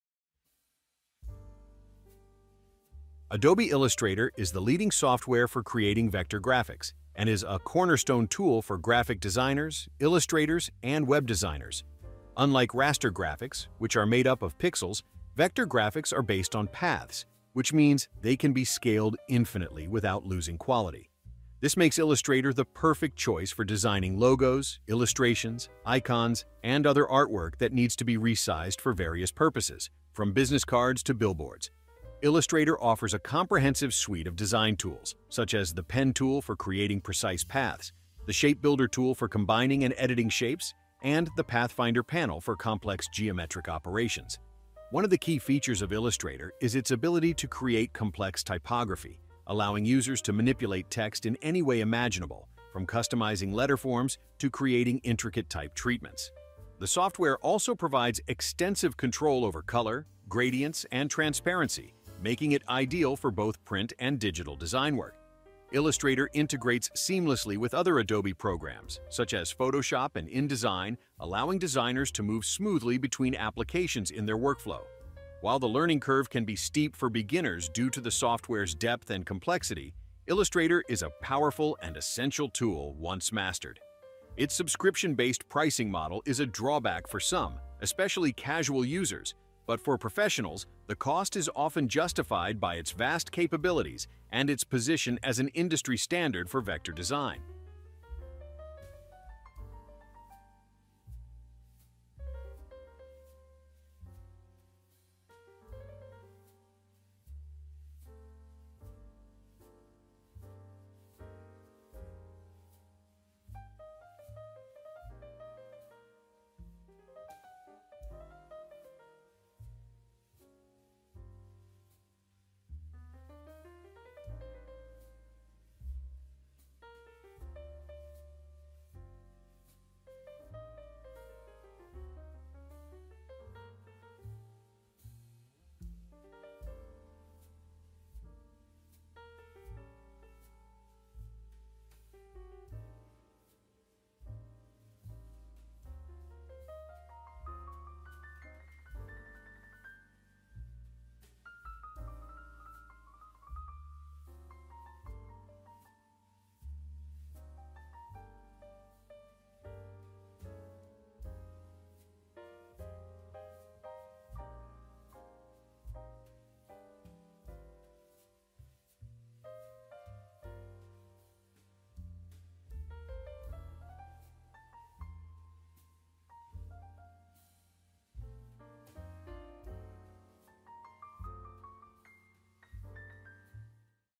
How to Download Adobe Illustrator | Latest Version Adobe Illustrator 2025
How to Download Adobe Illustrator 2025?

0:01 - What will be in the video
0:09 - Creative Cloud
0:43 - How to download Adobe Illustrator 2025
1:26 - Working with the program